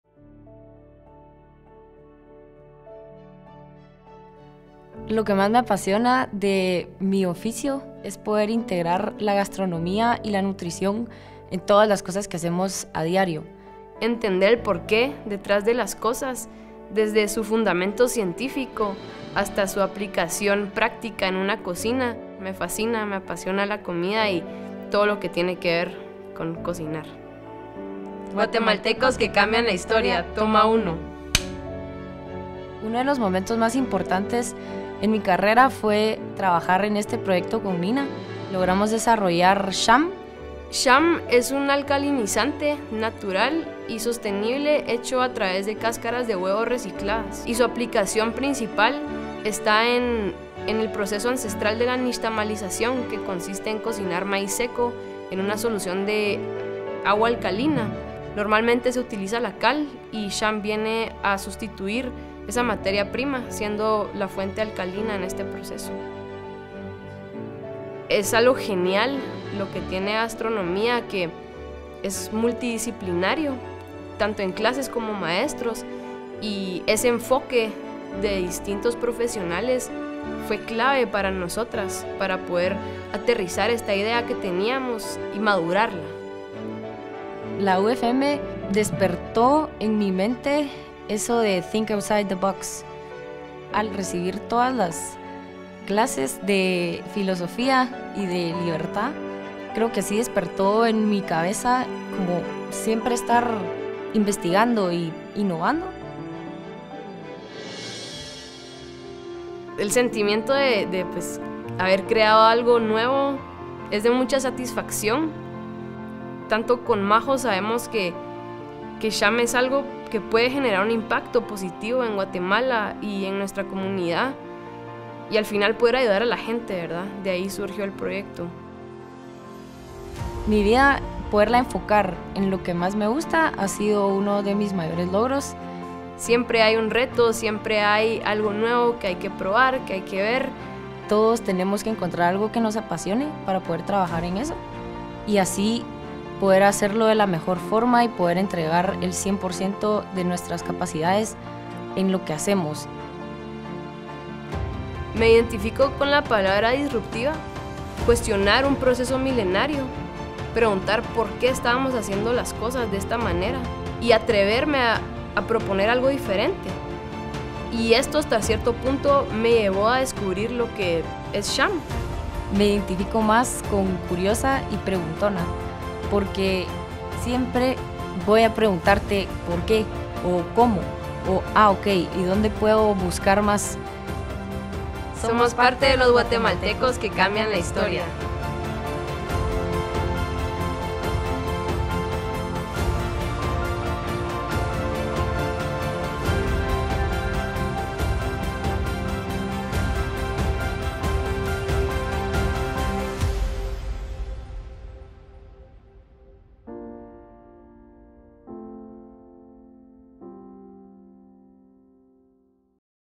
Alumni UFM Nina Ibargüen y María José Sánchez | Guatemaltecos que cambian la historia
Nina Ibargüen y María José Sánchez son fundadoras de Xam, emprendimiento que obtuvo el premio Nacional de Innovación 2022 en la categoría industrial por la Secretaría Nacional de Ciencia y Tecnología en Guatemala. Además, ganaron el tercer lugar en la primera edición del Desafío Bi en la categoría de innovación en alimentos, gastronomía o turismo, ideas y nuevos negocios organizado por el Centro de Emprendimiento Kirzner (KEC) de la Facultad de Ciencias Económicas en la Universidad Francisco Marroquín.

Ambas tienen una licenciatura en Gastronomía Nutricional y Empresarialidad por la Escuela de Nutrición en la Universidad Francisco Marroquín, Guatemala.

Nina Ibargüen y María José Sánchez forman parte de los guatemaltecos que cambian la historia.
¿Qué significa para ti ser alumni UFM?